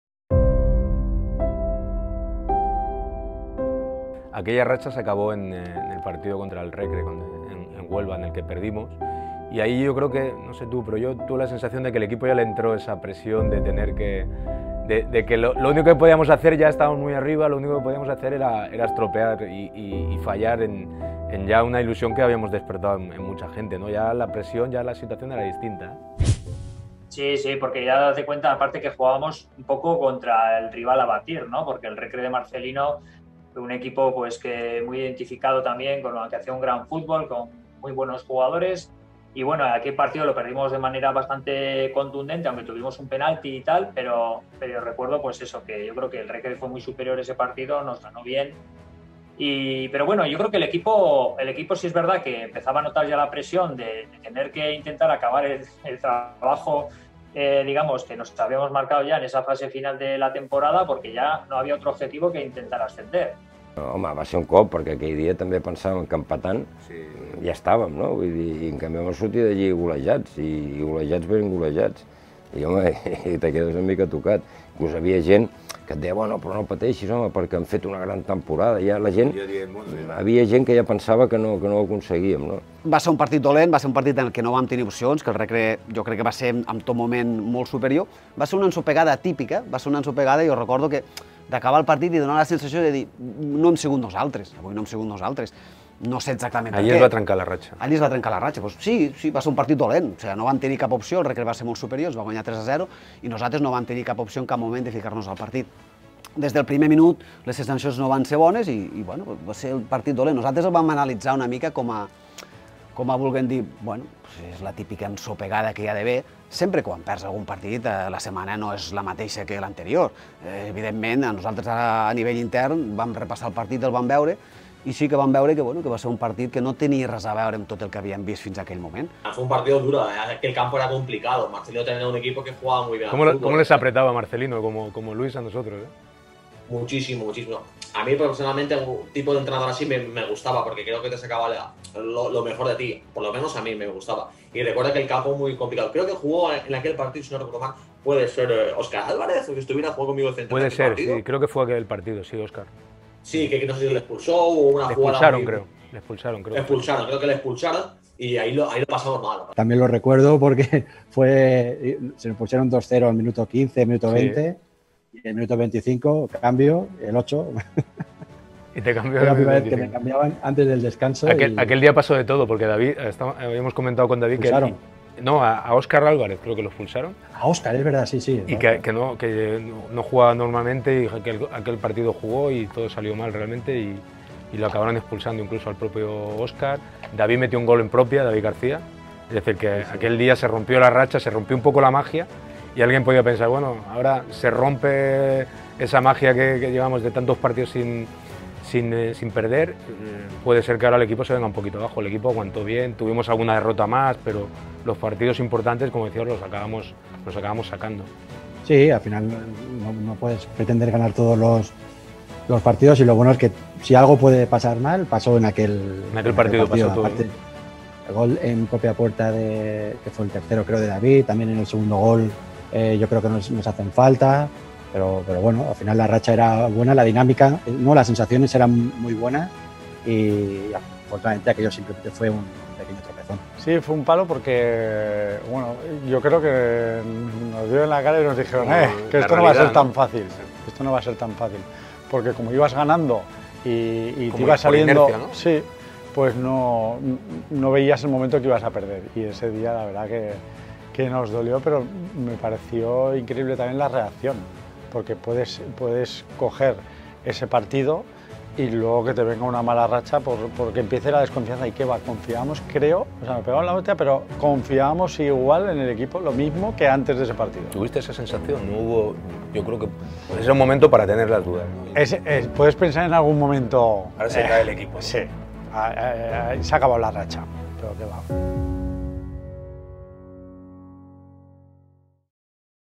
El final d’una ratxa gloriosa
Catorze jornades sense perdre. 36 punts de 42 possibles. Una segona volta immaculada que es va truncar amb una dura derrota contra el rival a batre, el Recreativo de Huelva. Després de tants partits sense perdre, es temia que l’equip abaixés els braços. Per sort, l’objectiu es va acabar complint.

3-0, Òscar Álvarez expulsat, un gol en pròpia de David Garcia… Un partit que va sortir malament, però que per sort només va interrompre la ratxa de partits seguits sense perdre. Un "Recre" on jugaven futbolistes que acabarien defensant la samarreta del Nàstic, al terreny de joc o als despatxos: Iago Bouzón, Juan Merino, Emilio Viqueira, Uche i Calle.

Al capítol d’avui tindrem els testimonis del mateix David Garcia, Ismael Irurzun, Josep Maria Andreu, Xavi Bartolo, Àngel Morales i Rubén Pérez.